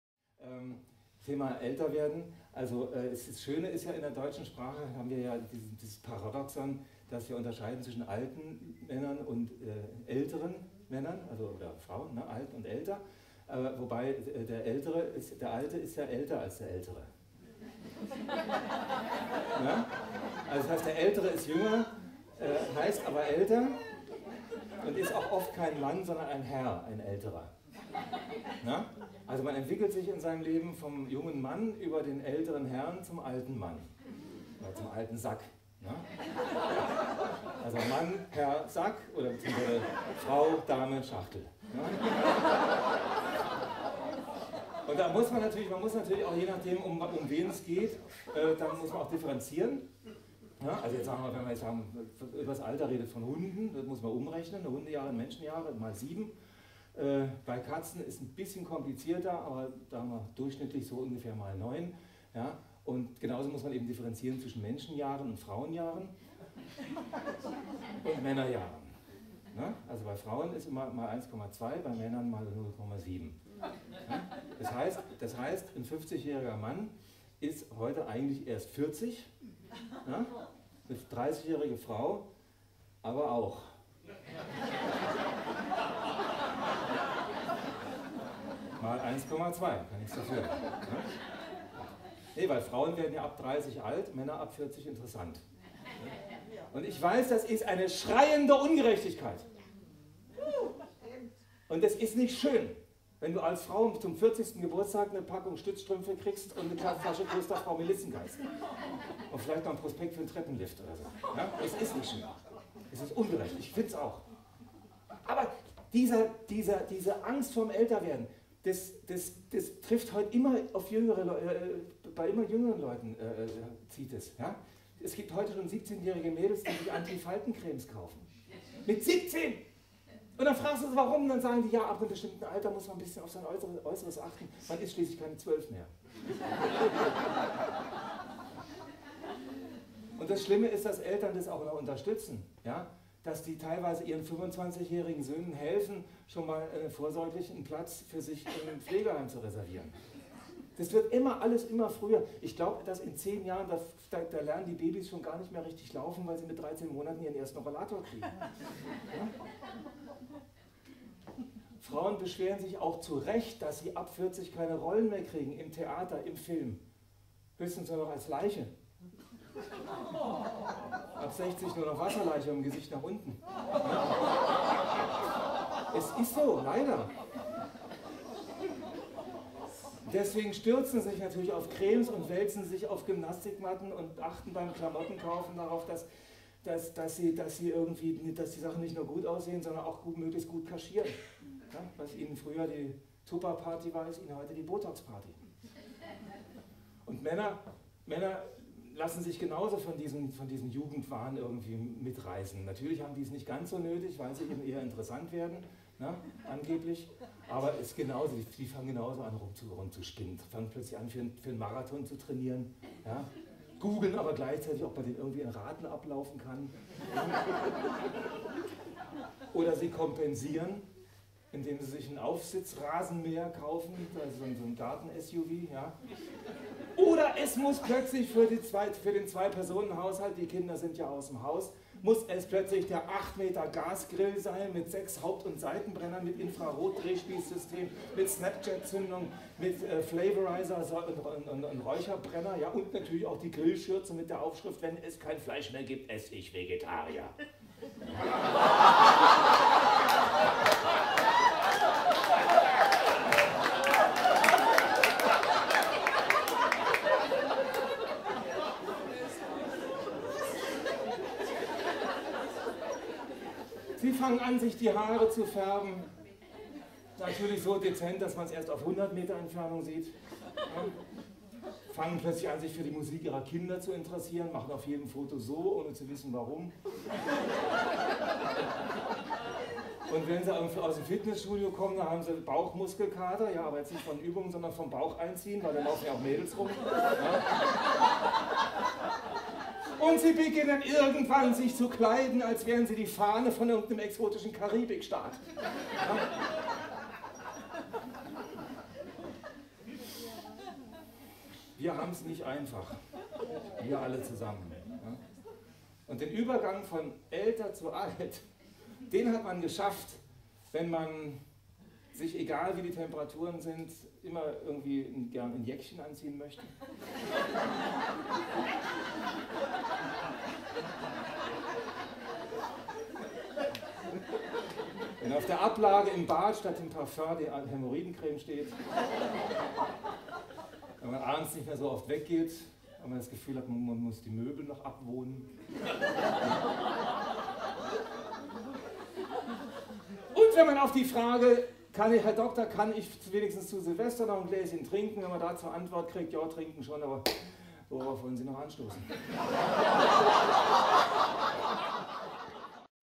Frank Sauer - Älter werden
Frank Sauer zeigt Ausschnitte aus seinem Programm "Scharf angemacht - die besten Rezepte für Beziehungssalat".